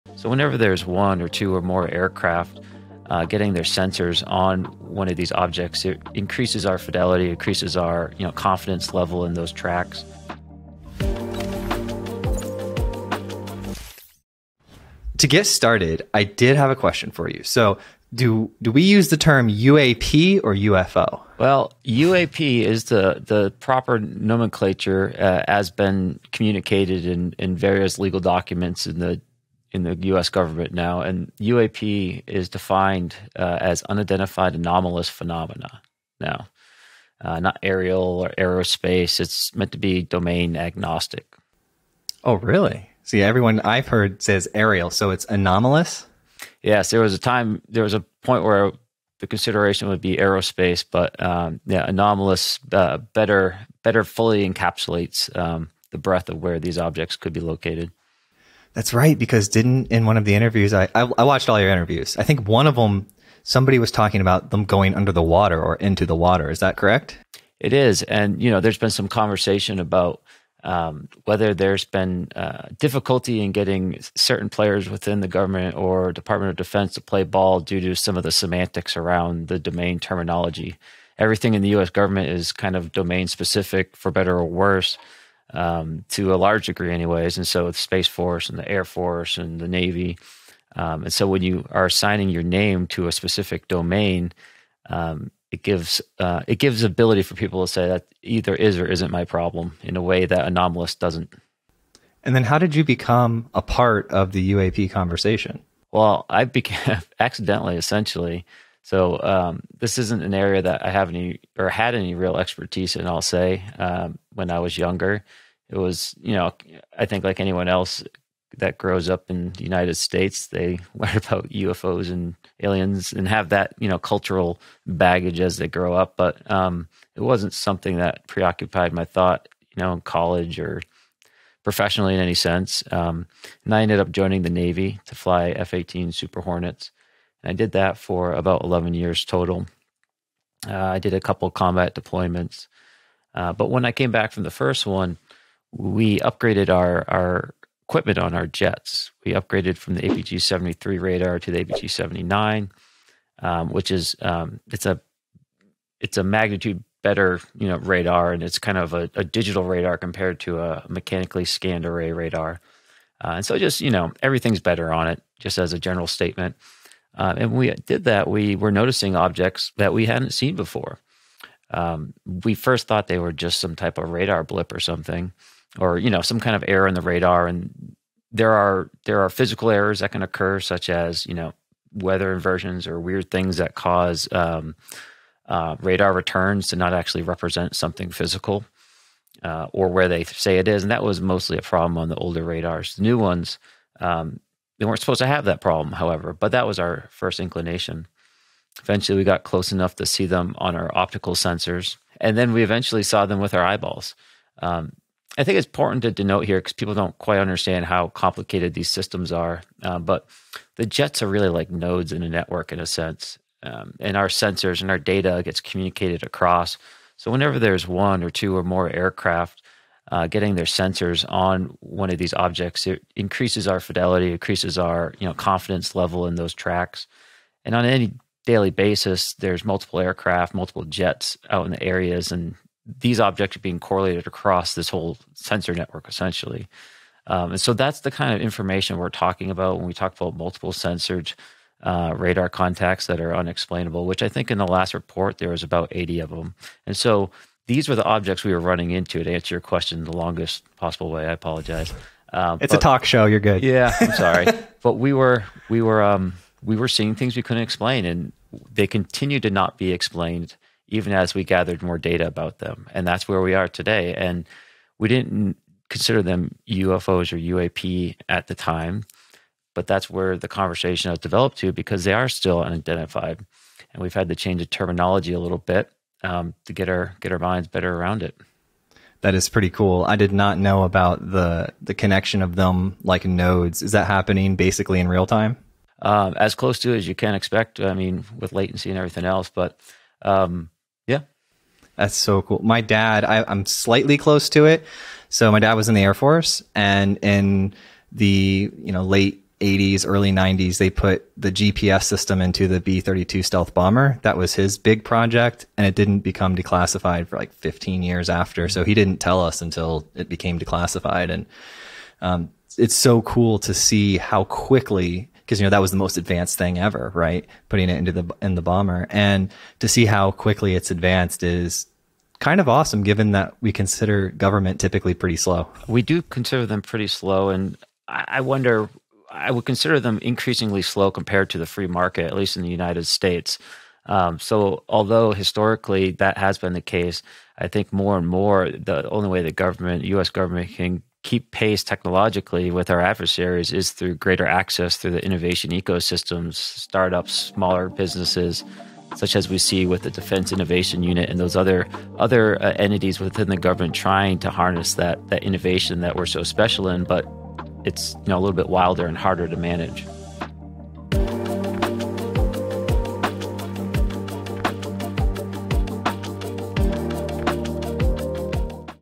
The Fighter Jet technology behind the UFO sightings
In this exclusive interview, Navy Fighter Pilot Ryan Graves discusses the fascinating technology behind the recent UFO sightings in Virginia. As one of the pilots who witnessed these strange objects firsthand, Graves provides unique insights into the advanced capabilities of these unidentified anomalous phenomena. From the cutting-edge sensors on his fighter jet to the complex maneuvers of the UFOs, Graves reveals the incredible feats of engineering that make these sightings so intriguing. Don't miss this rare opportunity to hear from a seasoned pilot and learn more about the mysteries of our skies.

All of this right here, right now, on the Modern CTO Podcast!

Produced by ProSeries Media.